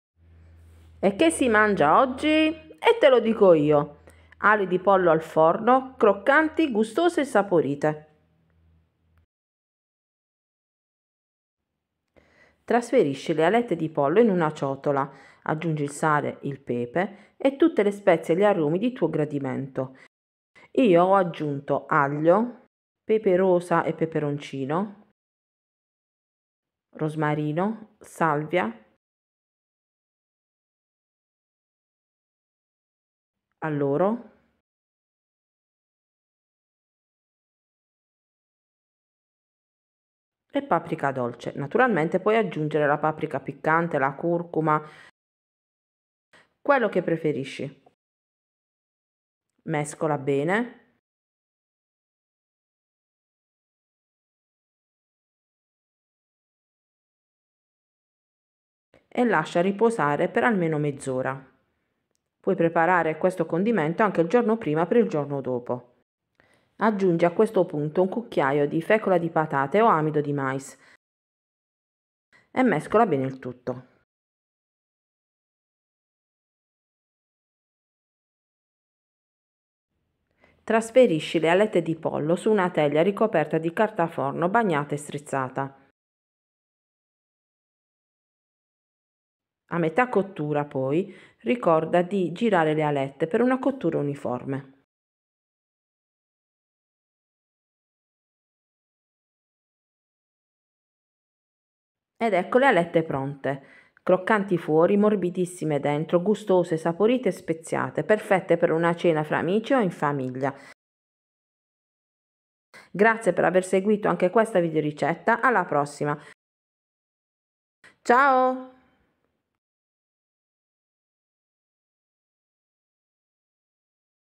ALI DI POLLO AL FORNO CROCCANTI
Le ali di pollo al forno croccanti o alette sono un secondo piatto gustoso e saporito facile e veloce da realizzare , perfette come cena o piatto unico per  tutta la famiglia.

Le alette al forno croccanti sono una idea sfiziosa per una cena in compagnia di amici o per trascorrere una serata in famiglia,  si tratta di un piatto che in genere piace a tutti. Da accompagnare con salse come  MAIONESE o KETCHUP ,  o con  PATATE AL FORNO  o con un contorno di  CAROTE E PISELLI IN PADELLA  o  FAGIOLINI IN UMIDO .

Per preparare le ali di pollo al forno basteranno pochi passaggi e pochi ingredienti per un piatto ricco e saporito ideale ,anche , per una cena o pranzo all’aperto  : alette di pollo,  sale e pepe , peperoncino ( se gradito) , paprika dolce o piccante , curcuma,  aglio,  rosmarino,  timo,  alloro ( naturalmente potete aggiungere tutti gli aromi e le spezie che preferite. In più, c’è un ingrediente segreto che vi permetterà di ottenere delle alette di pollo asciutte e croccanti : si tratta della fecola di patate o amido di mais!

Dopo aver preparato insieme il  POLLO ALLA BIRRA  e il  POLLO CON LE PATATE AL FORNO  ecco che arriva una delle ricette più gustose di tutte !

Forza! Ora andiamo in cucina , ci sono le ali di pollo croccanti al forno da preparare,  per la gioia di tutta la famiglia sempre dal cuore alle mani per te!